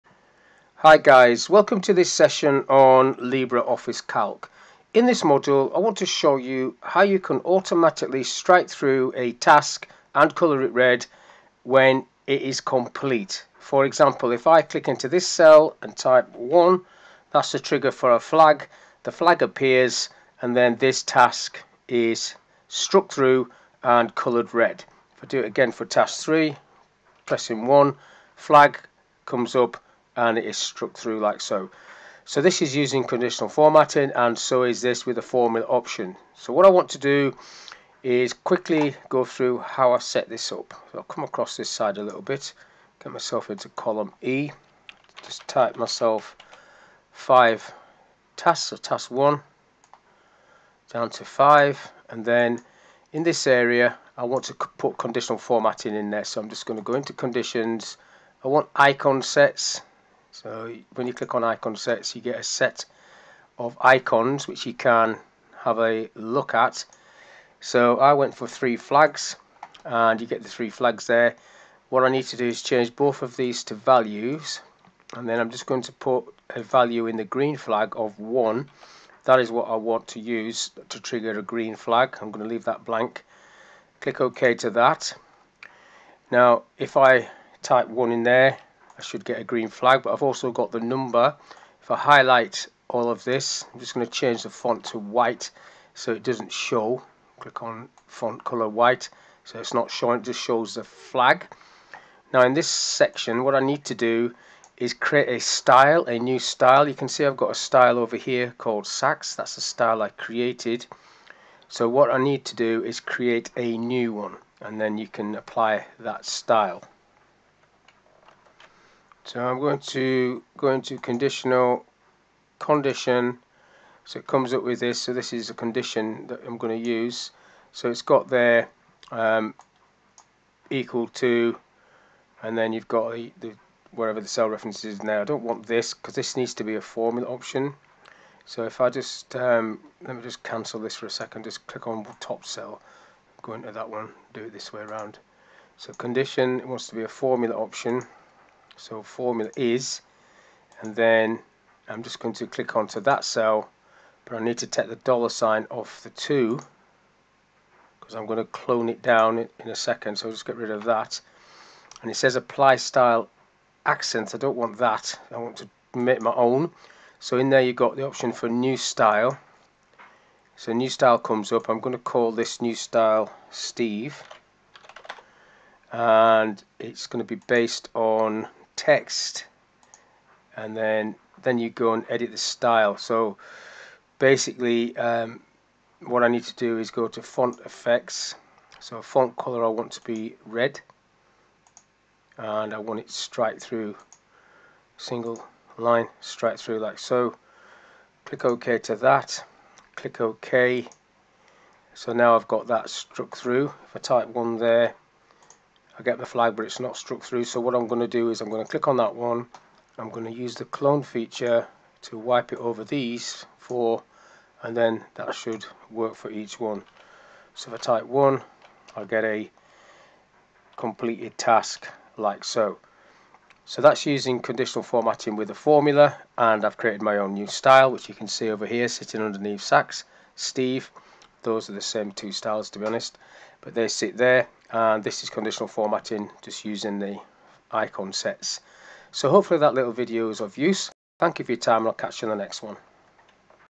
How to automatically mark task complete in LibreOffice Calc
Use conditional formatting to automatically mark tasks complete. Conditional formatting is a feature in LibreOffice Calc that allows you to apply different formats to cells based on certain criteria. For example, you can use conditional formatting to automatically mark tasks complete in a spreadsheet by changing the background colour or font style of the cells.

To use conditional formatting to mark tasks complete, you need to follow these steps:

1. Select the cells that contain the tasks you want to mark as complete. You can select a single cell, a range of cells, or the entire column.
2. Go to Format - Conditional - Condition from the menu bar. This will open the Conditional Format dialog box.
3. In the Conditional Format dialog box, click on Add to create a new condition. You can create up to four conditions for each cell range.
4. In the Condition 1 section, choose Cell value from the first drop-down list, choose equal to from the second drop-down list, and enter 1 (or any other word you use to indicate completion) in the text box. This means that the condition will be met when the cell value is equal to 1.
5. In the Apply Style section, click on the More button to open the Cell Style dialog box. Here you can choose any style you want to apply to the cells that meet the condition. For example, you can choose a green background colour and a check mark font style to indicate completion. Click OK to close the Cell Style dialog box and return to the Conditional Format dialog box.
6. Click OK to close the Conditional Format dialog box and apply the conditional formatting to the selected cells. You will see that the cells that contain 1 will change their format according to the style you chose.
7. To mark a task as complete, simply enter 1 in the corresponding cell and press Enter. The cell will automatically change its format to indicate completion.
8. To remove the conditional formatting, select the cells that have it applied and go to Format - Conditional - Clear from the menu bar.